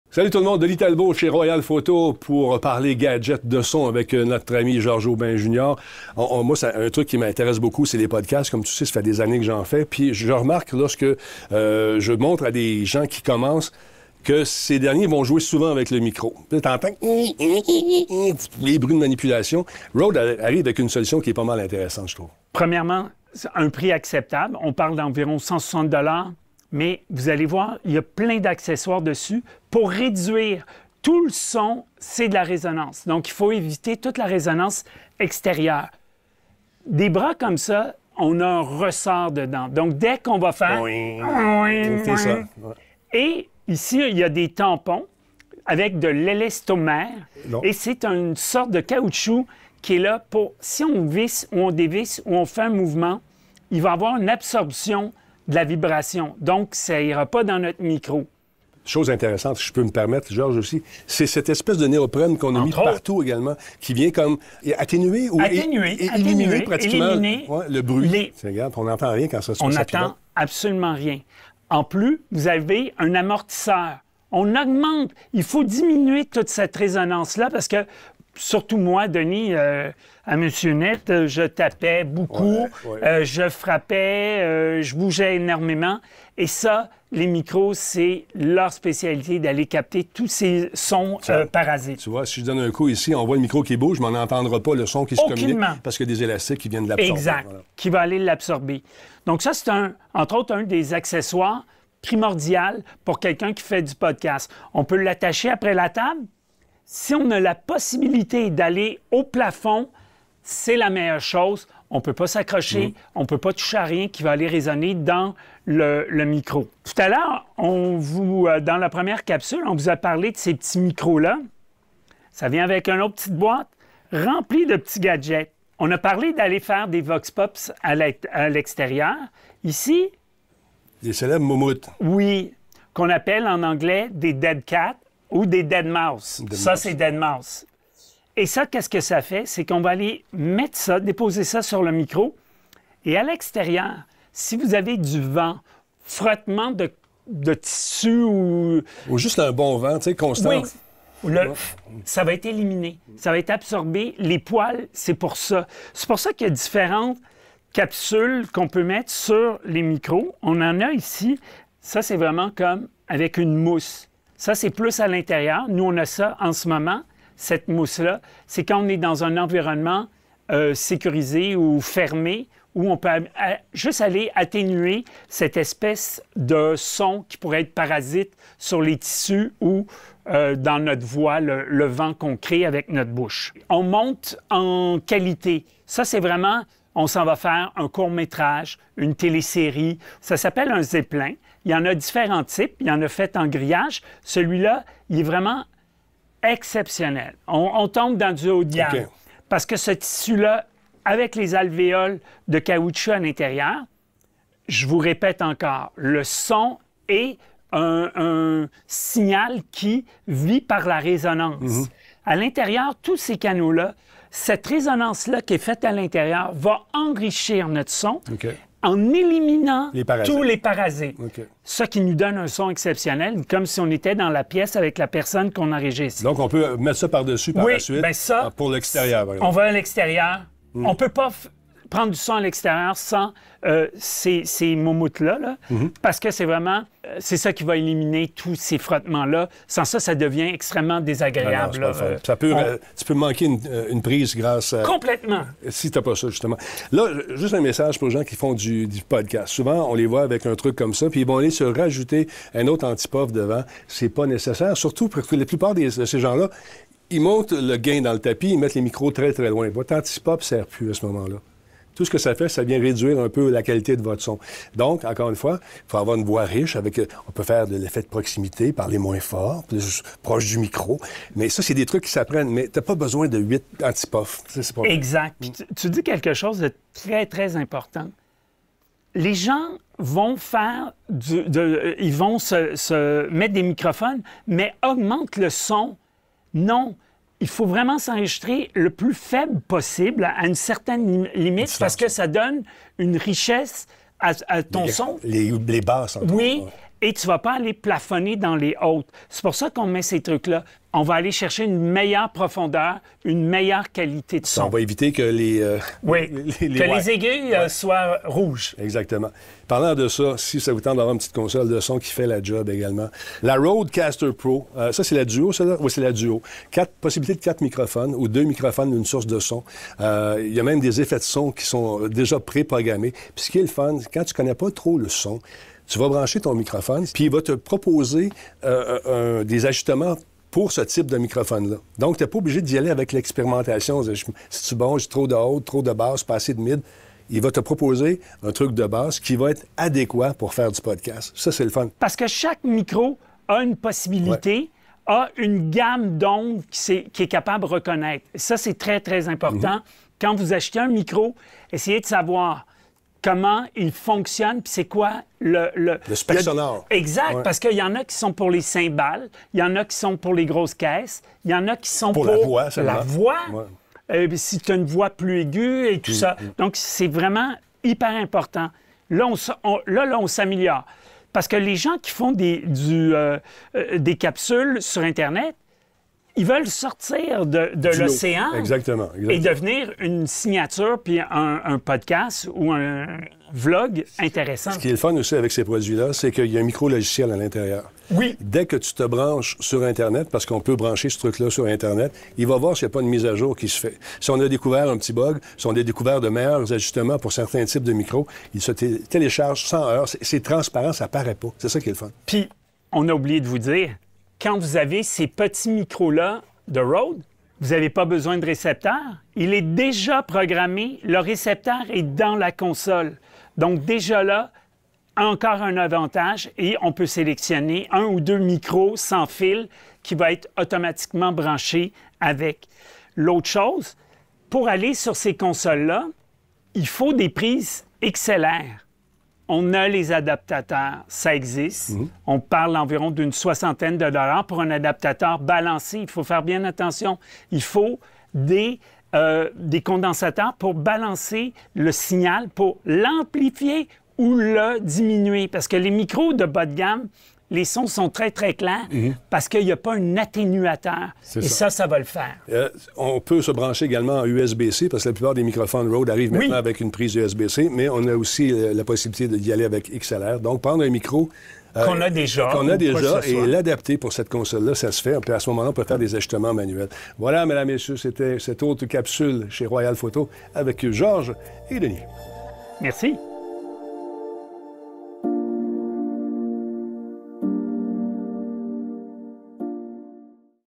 Chronique Info Royal Photo - animée par Denis Talbot - #3 les accessoires audio
Trépieds, moumoute, Zeppelin et autres écrans... que ce soit pour votre podcast, vlog, tournage, cinéma-TV ou tout autre projet audio ; trouvez le bon accessoire pour votre situation grâce aux conseils avisés de Denis Talbot et Georges Aubin Jr. Vous décuplerez le potentiel de votre microphone.

Tripods, wind shields, Zeppelin and other screens... whether for your podcast, vlog, filming cinema-TV or any other audio project. Find the right accessory for your situation thanks to sound advice from Denis Talbot and Georges Aubin Jr. You'll get more out of your microphone.

Photo vignette : Cyrille Cambien
Équipe tournage vidéo : spécialistes Royal Photo